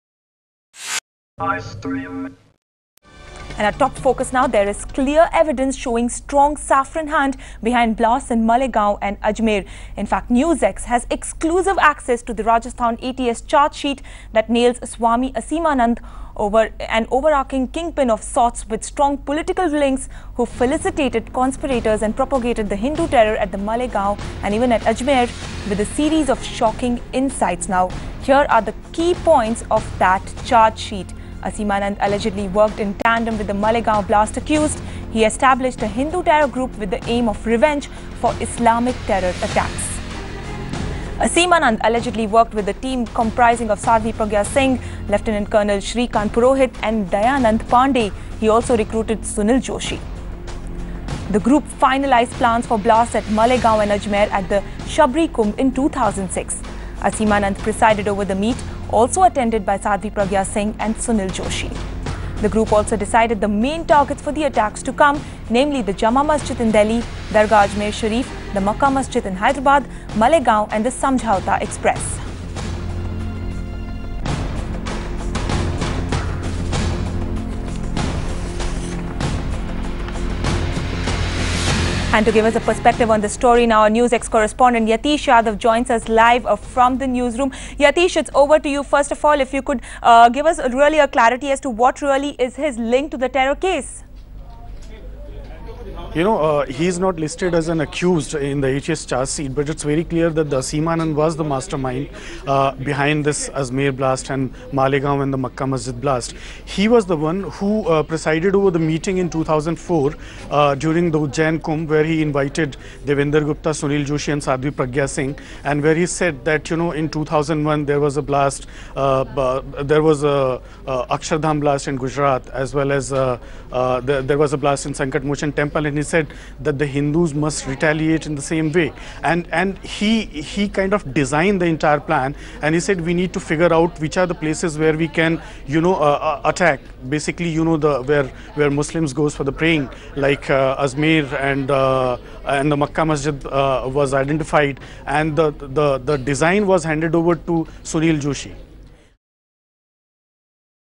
Blast probe leads to Hindu group
ETS chargesheet nails Swamy Aseemanand who allegedly worked in tandem with the Malegaon blast accused ...

Source: News X.